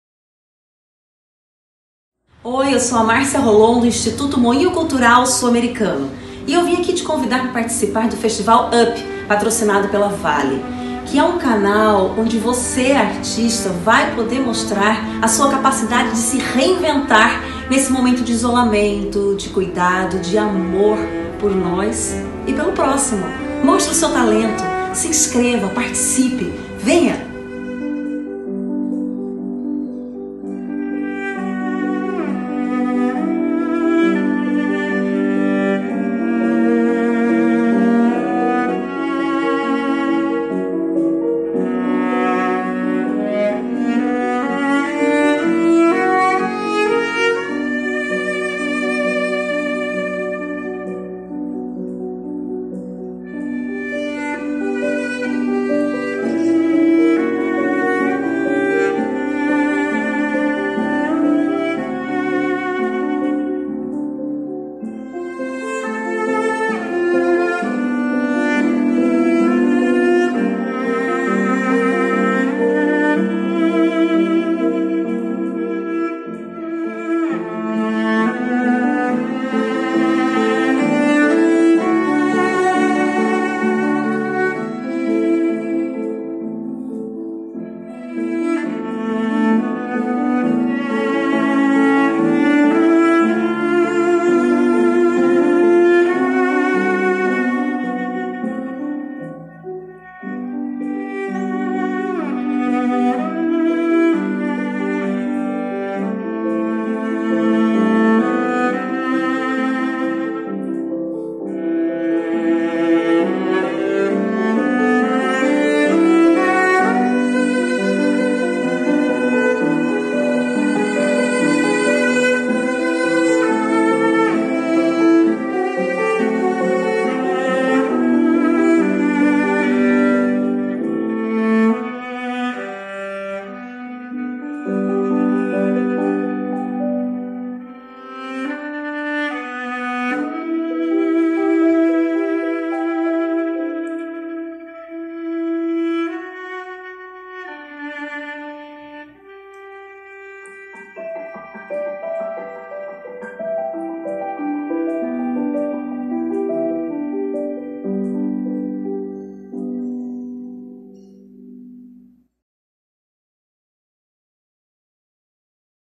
Festival Up
Convite da nossa Diretora Executiva Marcia Rolon.

Vale e Instituto Ekloos criam festival para apoiar artistas autônomos durante a pandemia do coronavírus